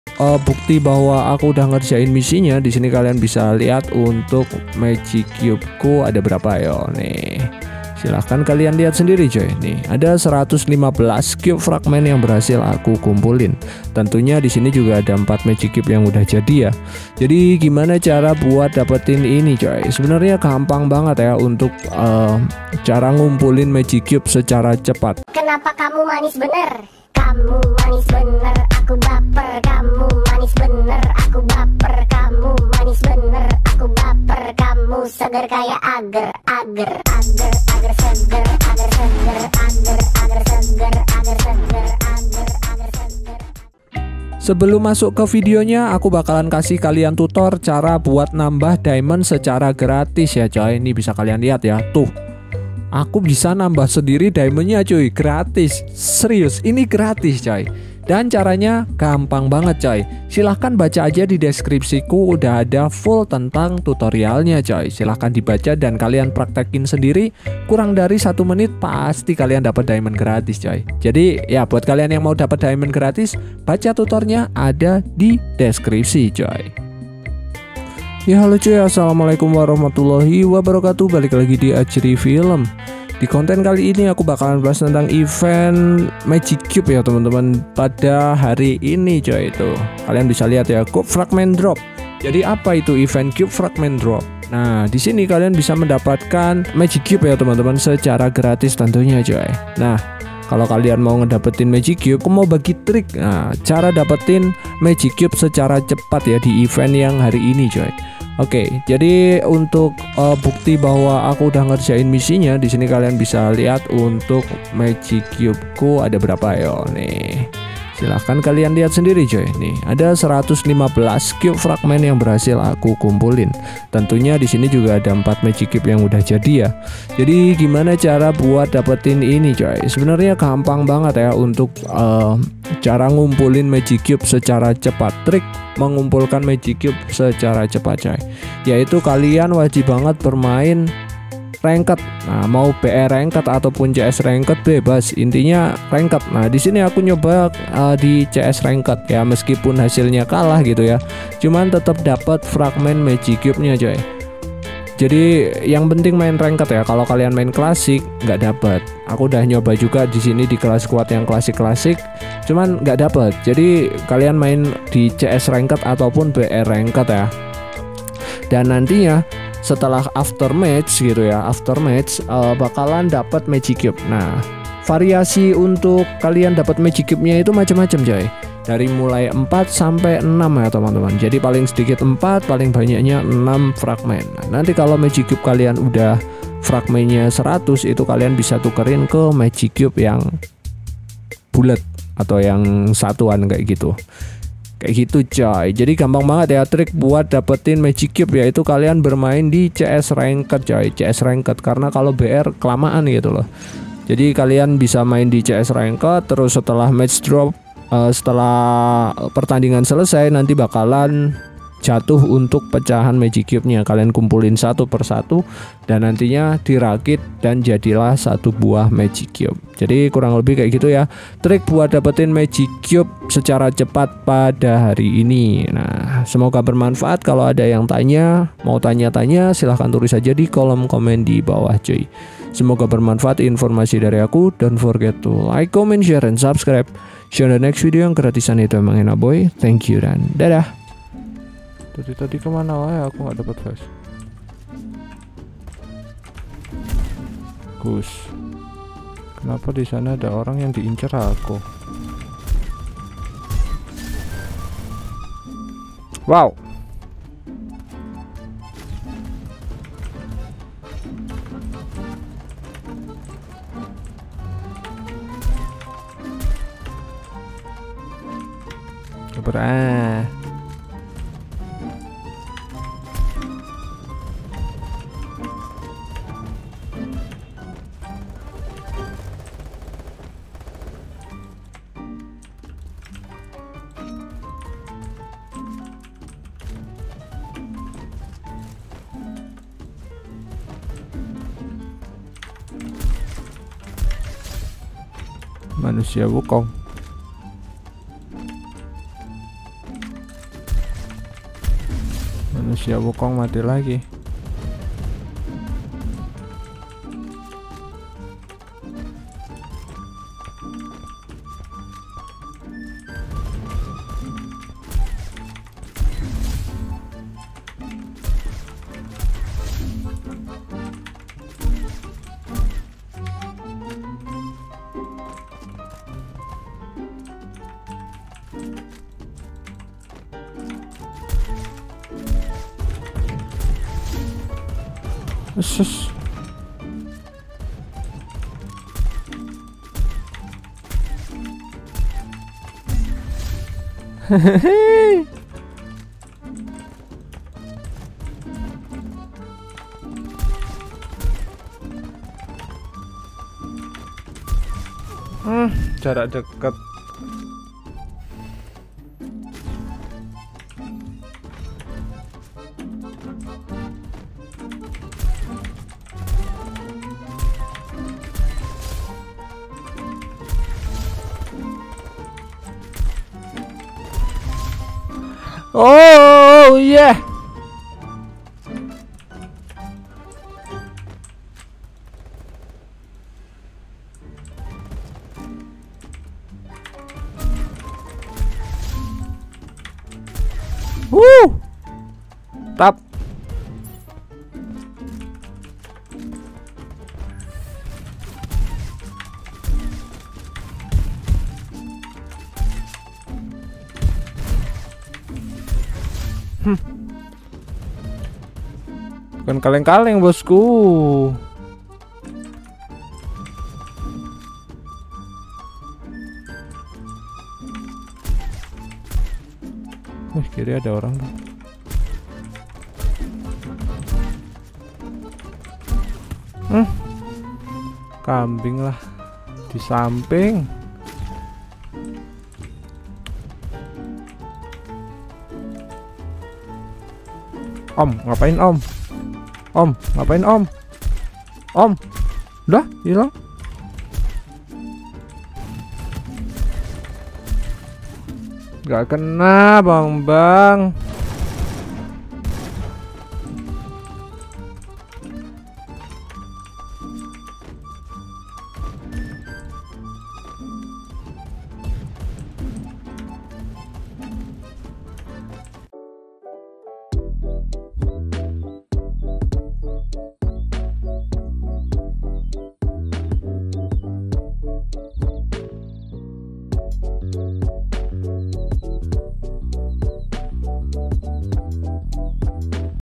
TRIK CEPAT DAPET MAGIC CUBE !! CARA CEPAT KUMPULKAN BANYAK FRAGMENT MAGIC CUBE , EVENT BARU HARI INI
Bagi yang mau dukungan donasi or support channel ini biar lebih berkembang bisa
atau
• Gift Bundle/Diamond

Mabar atau Join live bisa gabung di discord